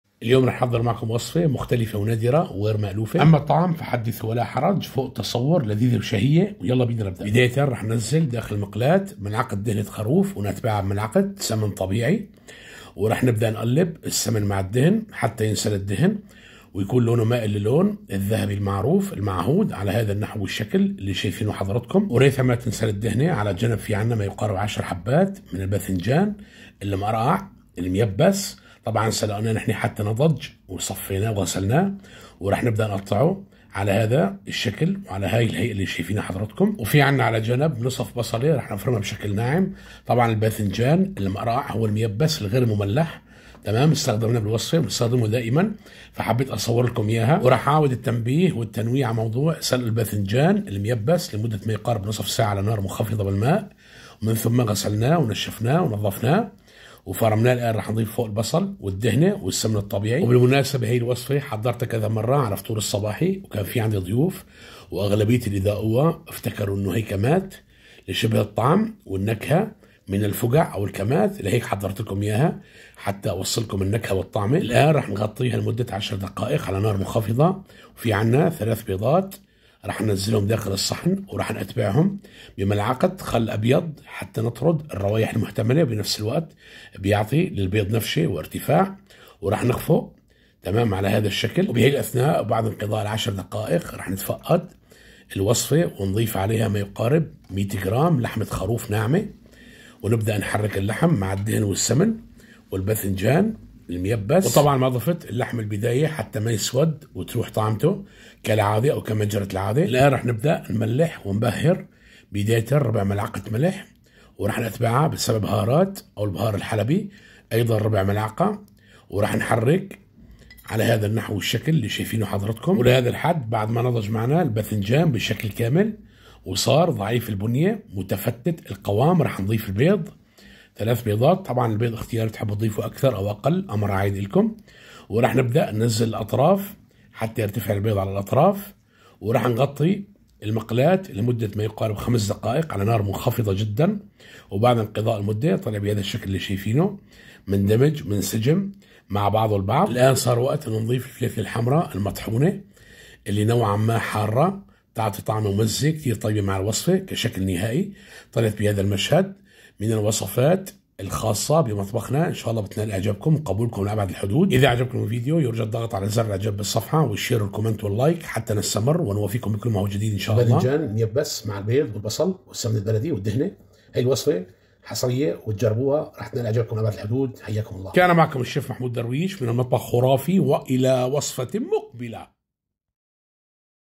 من وصفات الشيف الحصرية ✅️علقوا بتم وشاركوا الفيديو لطفا 🌹
من وصفات الشيف الحصرية ✅️
علقوا بتم وشاركوا الفيديو لطفا 🌹

المكونات والمقادير:

دهنة خروف ملعقة
سمن طبيعي ملعقة
باذنجان مقرقع عشر حبات حجم صغير مسلوق لدرجة النضوج ومغسول ومجفف
نصف بصلة او بصلة صغيرة
لحمة خروف ناعمة 100 حرام
ثلاث بيضات
ملعقة خل ابيض صغيرة
ملح ربع ملعقة
بهار حلبي ربع ملعقة
فليفلة حمرة ناعمة
والف هنا وعافية

طريقة التحضير:

ستجدونها كاملة مفصلة بالفيديو اعلاه ☝️